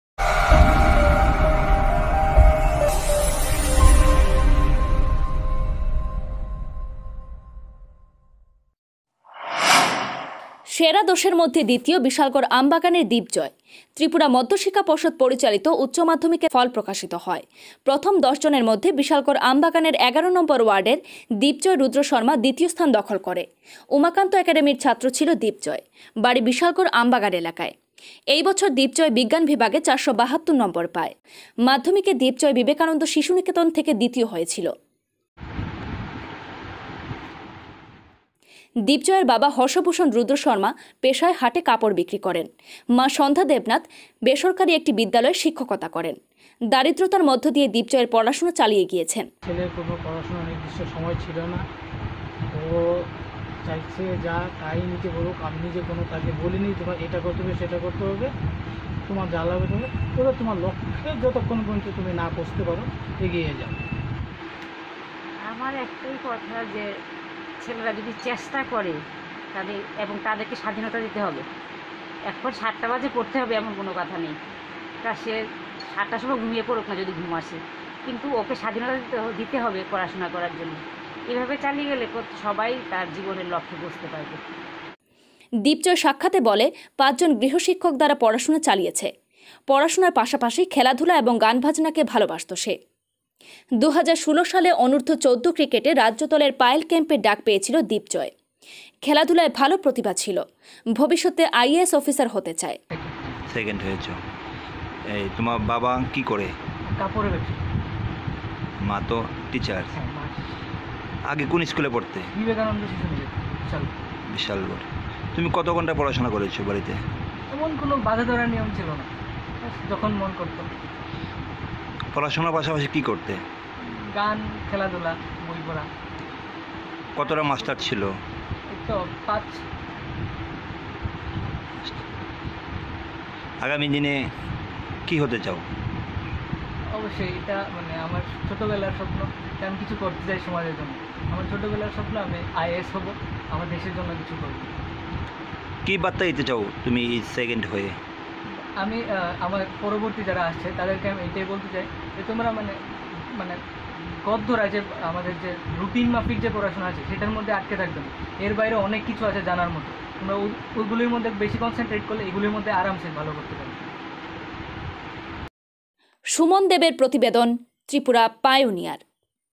উচ্চমাধ্যমিক ২০২০: ৪৭২ নম্বর পেয়ে বিজ্ঞান বিভাগে ২য় স্থান দখল করল বিশালগড়ের দীপজয়, স্বপ্ন IAS
বিশালগড় সংবাদ